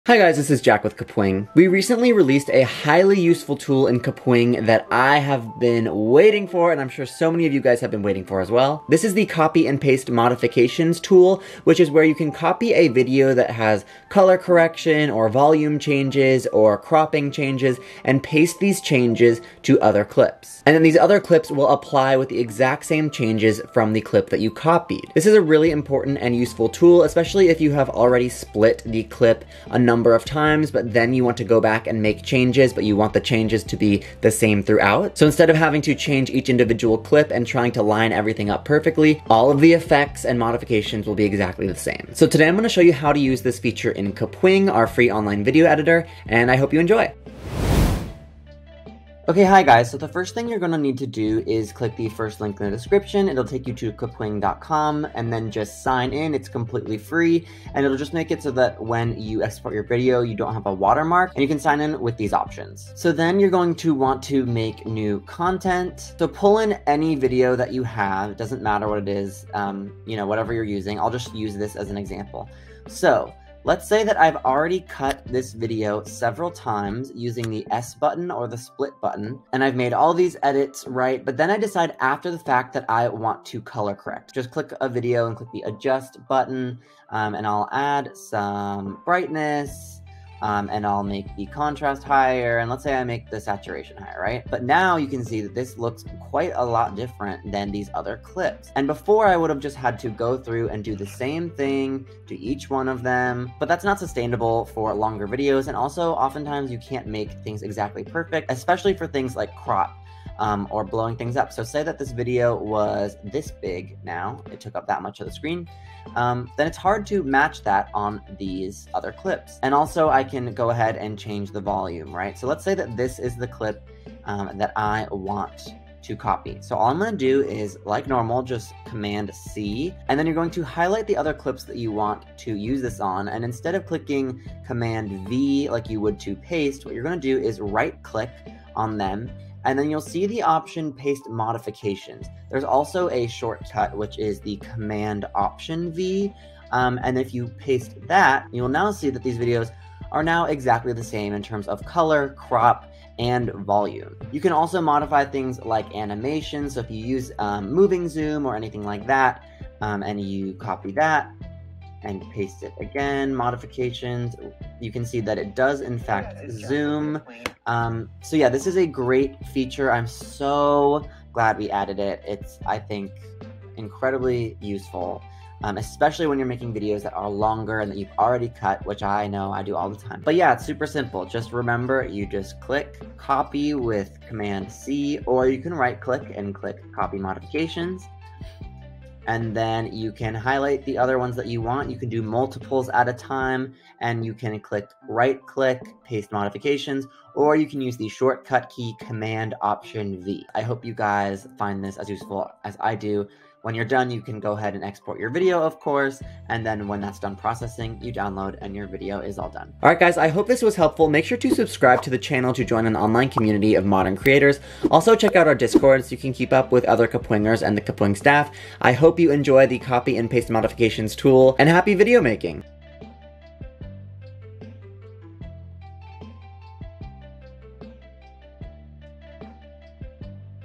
How to Copy and Paste Video Effects in Kapwing
Kapwing's new Copy and Paste Modifications feature just made video editing a lot easier! If you've ever edited videos and needed to color correct, crop, or change the volume and effects of all clips to be exactly the same, you are going to love this feature. Today we will show you how to do this on Kapwing, our free online editor, in just a few clicks.

Timestamps:
0:00 Copy and Paste Modifications Overview
0:47 How to Copy and Paste Modifications
3:30 Outro